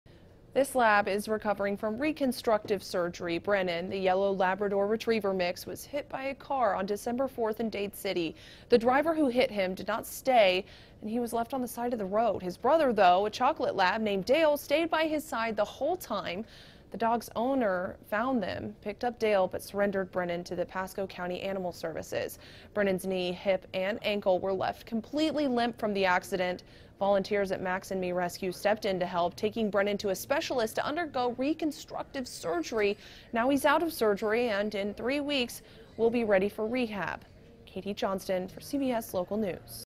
Dog Hit By Car Undergoes Surgery After Brother Stays By His Side Waiting For Help
A Labrador Retriever is recovering nicely after undergoing reconstructive surgery for injuries suffered when he was hit by a car and left on the side of the road. Katie Johnston reports.